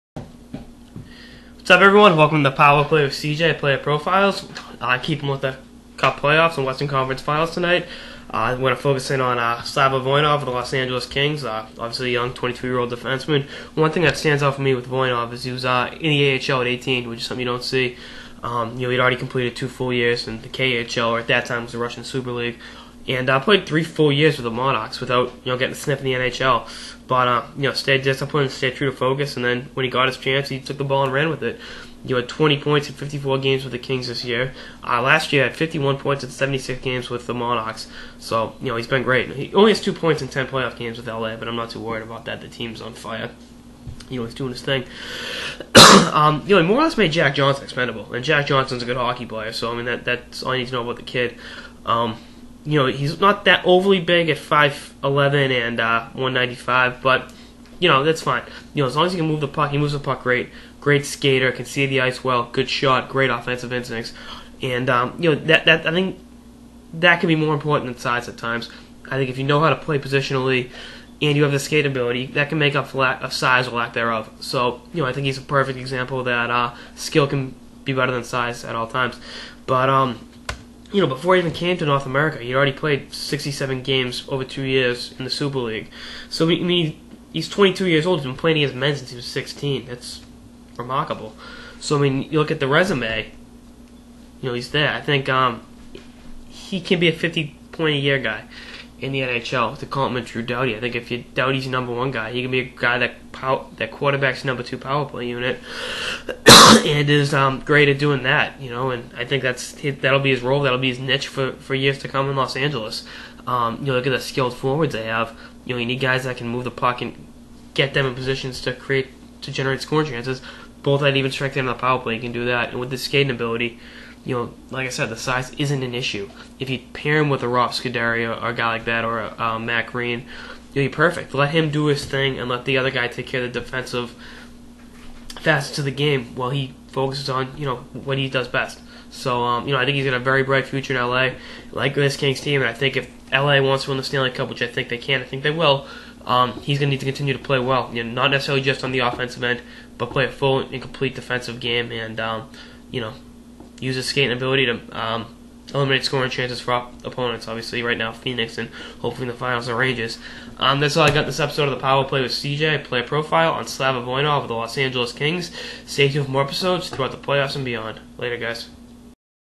Power Play with CJ Player Profile: Slava Voynov
CJ dishes on the skilled Russian defenseman playing his trade with the Kings and what the youngster offers fans in Hollywood